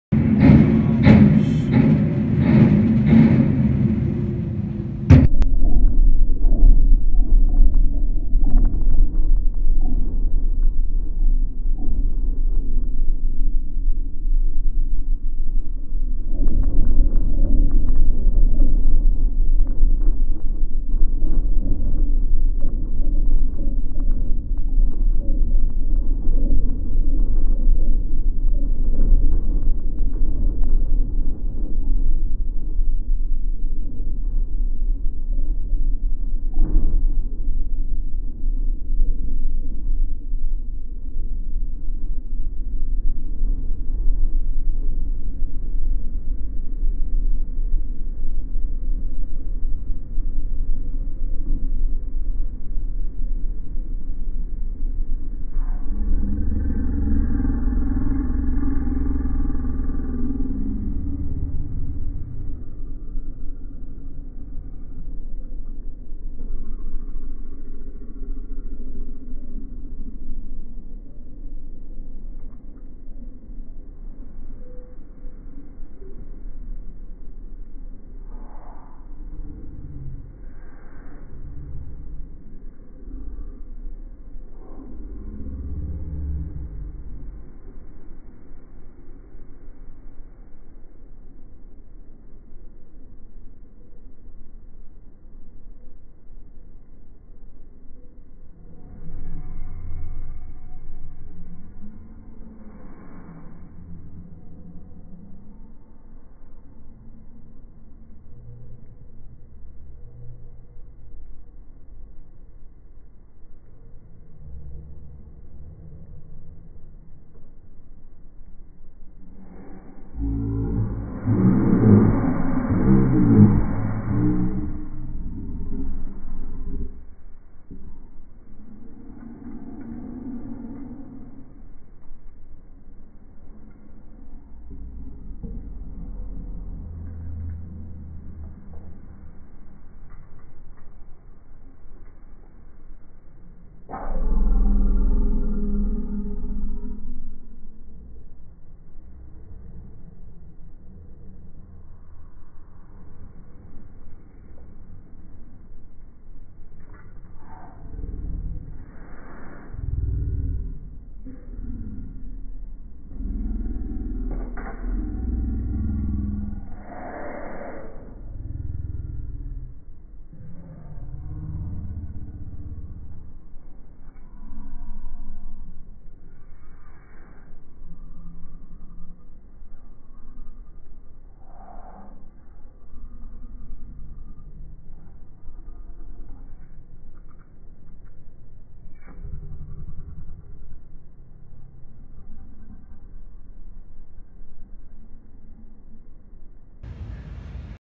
Building Implosion slow motion
Ashbel Smith Hall downtown Austin, Tx